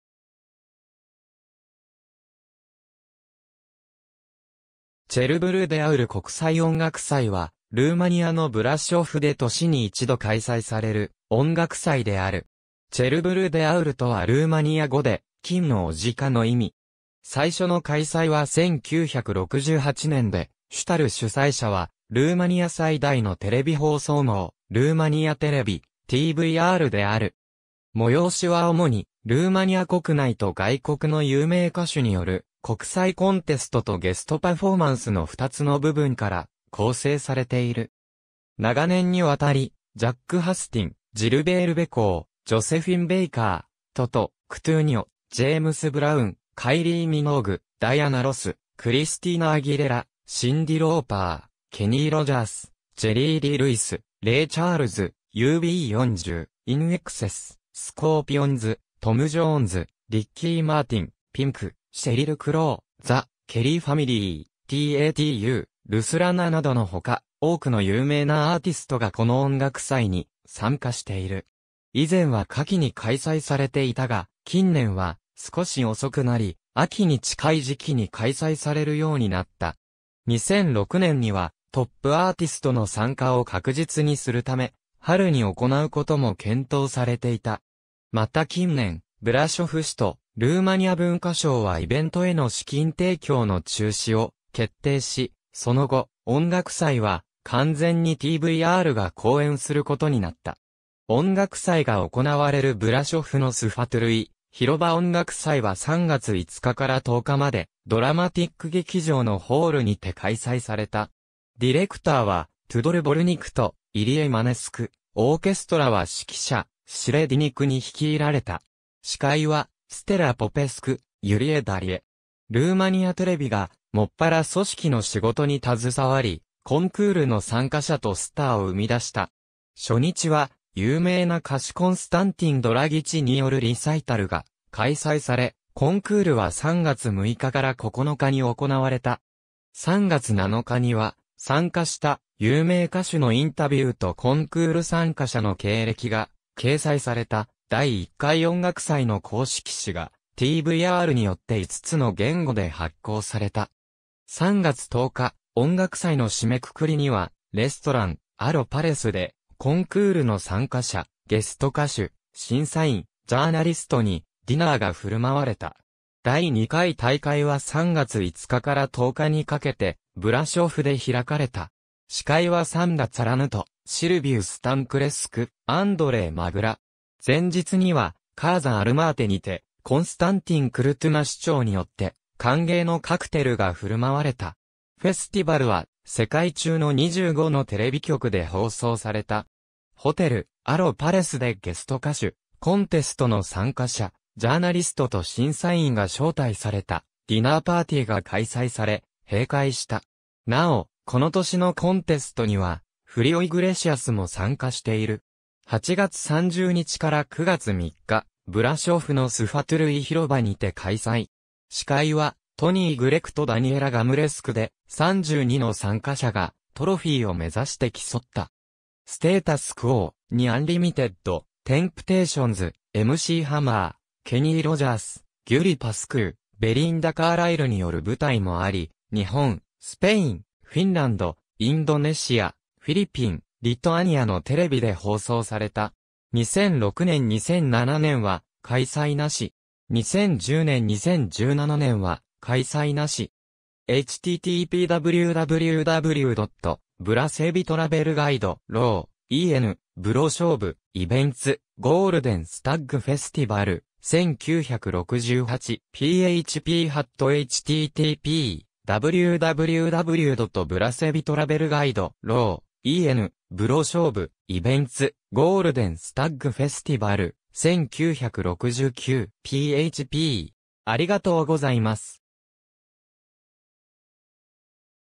チェルブル・デ・アウル国際音楽祭
チェルブル・デ・アウル国際音楽祭 (ルーマニア語: Festivalul Internaţional „Cerbul de Aur”、または単にCerbul de Aur) は、 ルーマニアの ブラショフで年に一度開催される音楽祭である。
「チェルブル・デ・アウル」とはルーマニア語で「金の牡鹿」の意味。
最初の開催は1968年で、主たる主催者はルーマニア最大のテレビ放送網、ルーマニアテレビ (ルーマニア語: Televiziunea Română)、TVR）である。
催しは主に、ルーマニア国内と外国の有名歌手による、国際コンテストとゲストパフォーマンスの二つの部分から構成されている。
長年にわたり、 Jacques Hustin 、ジルベール・ベコー、ジョセフィン・ベーカー、トト・クトゥーニョ、ジェームス・ブラウン、カイリー・ミノーグ、ダイアナ・ロス、クリスティーナ・アギレラ、シンディ・ローパー、ケニー・ロジャース、ジェリー・リー・ルイス、レイ・チャールズ、UB40、INXS、スコーピオンズ、トム・ジョーンズ、リッキー・マーティン、ピンク、シェリル・クロウ、The Kelly Family、t.A.T.u.、ルスラナなどの他、多くの有名なアーティストがこの音楽祭に参加している。
以前は夏期に開催されていたが、近年は少し遅くなり、秋に近い時期に開催されるようになった。
2006年には、トップアーティストの参加を確実にするため、春に行う事も検討されていた。
また近年、ブラショフ市とルーマニア文化省はイベントへの資金提供の中止を決定し、その後、音楽祭は完全にTVRが後援することになった。
音楽祭が行われるブラショフのスファトゥルイ広場 音楽祭は3月5日から10日まで、ドラマティック劇場（Teatrul Dramatic）のホールにて開催された。
ディレクターはトゥドル・ヴォルニクとイリエ・マネスク、オーケストラは指揮者シレ・ディニクに率いられた。
司会はステラ・ポペスク、ユリエ・ダリエ。
ルーマニアテレビが専ら組織の仕事に携わり、コンクールの参加者とスターを生み出した。
初日は、有名な歌手コンスタンティン・ドラギチによるリサイタルが開催され、コンクールは3月6日から9日に行われた。
3月7日には、参加した有名歌手のインタビューとコンクール参加者の経歴が掲載された、第1回音楽祭の公式誌がTVRによって5つの言語で発行された。
3月10日、音楽祭の締めくくりにはレストラン「Aro Palace」（当時の名称は「Carpaţi」）で、コンクールの参加者、ゲスト歌手、審査員、ジャーナリストにディナーが振舞われた。
第2回大会は3月5日から10日にかけて、ブラショフで開かれた。
司会はサンダ・ツァラヌとシルヴュ・スタンクレスク（舞台）、アンドレイ・マグラ（ラジオ）。
前日には、「Casa Armatei」にて、コンスタンティン・クルトゥナ市長によって歓迎のカクテルが振舞われた。
フェスティバルは世界中の25のテレビ局で放送された。
ホテル「Aro Palace」でゲスト歌手、コンテストの参加者、ジャーナリストと審査員が招待されたディナーパーティーが開催され、閉会した。
なお、この年のコンテストにはフリオ・イグレシアスも参加している。
8月30日 - 9月3日、ブラショフのスファトゥルイ広場にて開催。
司会はトニー・グレクとダニエラ・ガムレスクで、32の参加者がトロフィーを目指して競った。
ステイタス・クォー、2 アンリミテッド、テンプテーションズ、M.C.ハマー、ケニー・ロジャース、Gyuri Pascu、ベリンダ・カーライルによる舞台もあり、日本、スペイン、フィンランド、インドネシア、フィリピン、リトアニアのテレビで放送された。
2006年・2007年は開催なし。
2010年・2017年は開催なし。